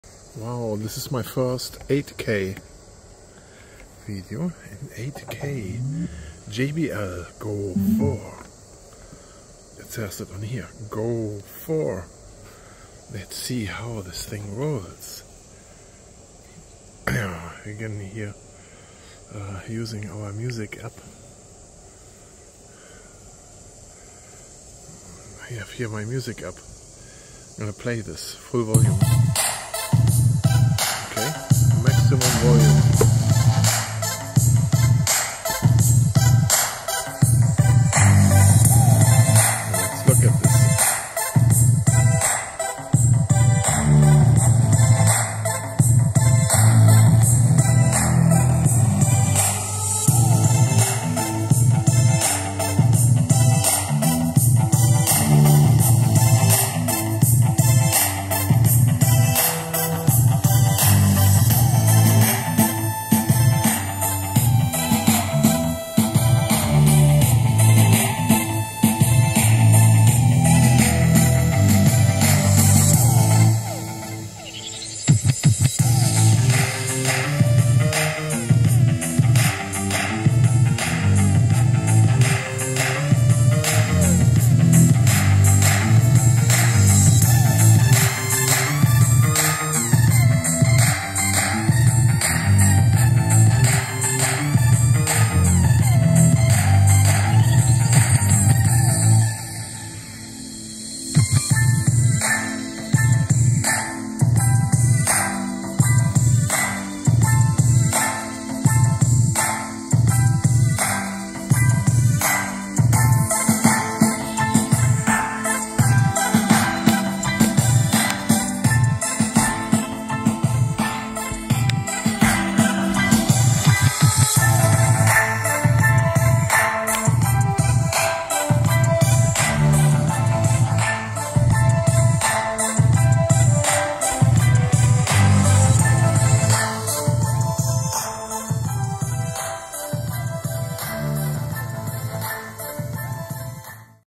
JBL GO4 Extreme Bass in 8K
My first upload in 8K. Hope it works out well. Here it is the JBL GO4.
Showing some extreme bass. The drivers are bending. Have fun!